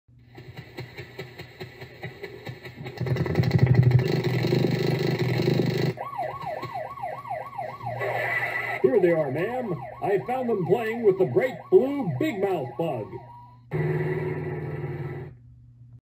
More Bugs in Boxes: I found them playing with a bright blue big Mouth Bug!
The scarlet Strawberry Bug was fanning her Wings to blow her Kids away and off to play with the bright blue big Mouth Bug that hadn’t seen and then the Police sent her Kids back to their Mother.